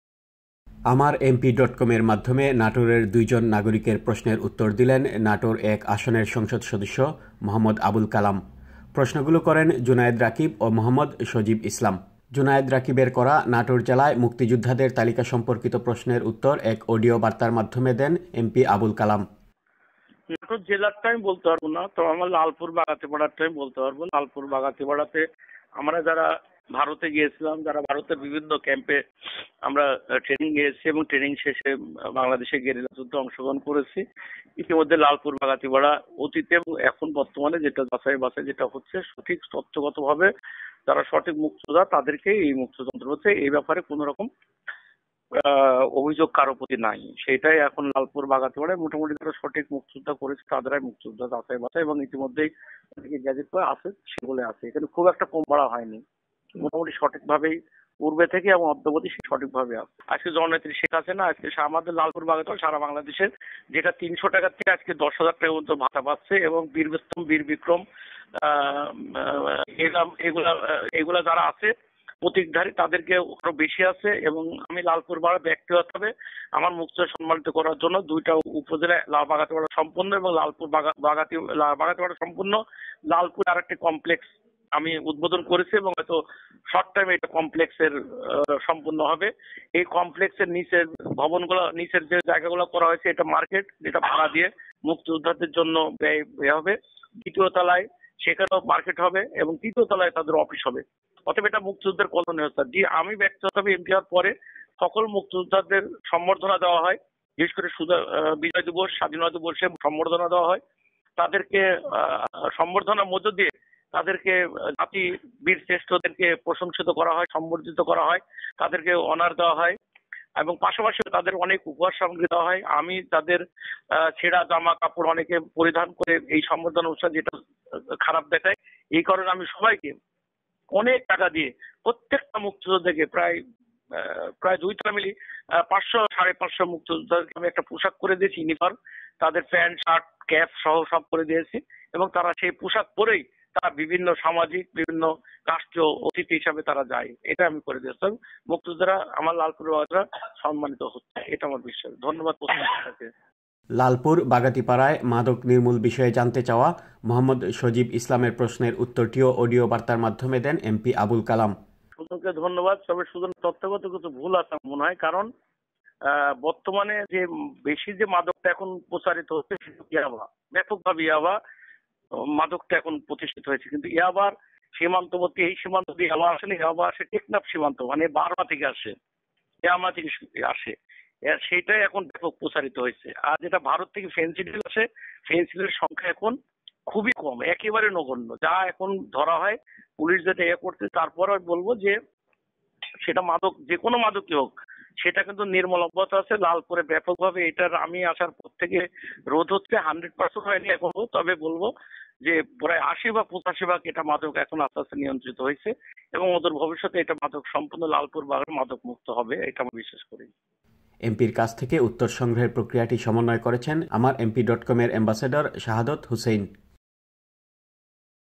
AmarMP: Reply from Md. Abul Kalam
আমার এমপি ডট কমের মাধ্যমে নাটোরের দুইজন নাগরিকের প্রশ্নের উত্তর দিলেন নাটোর ১ আসনের সংসদ সদস্য মোঃ আবুল কালাম। প্রশ্নগুলো করেন জুনায়েদ রাকিব ও মোঃ সজীব ইসলাম। জুনায়েদ রাকিবের করা নাটোর জেলায় মুক্তিযোদ্ধাদের তালিকা সম্পর্কিত প্রশ্নের উত্তর এক অডিও বার্তার মাধ্যমে দেন এমপি আবুল কালাম।

লালপুর বাগাতিপাড়ায় মাদক নির্মুল বিষয়ে জানতে চাওয়া মোঃ সজীব ইসলামের প্রশ্নের উত্তরটিও অডিও বার্তার মাধ্যমে দেন এমপি আবুল কালাম।

এমপির কাছ থেকে উত্তর সংগ্রহের প্রক্রিয়াটি সমন্বয় করেছেন শাহাদাত হুসেইন।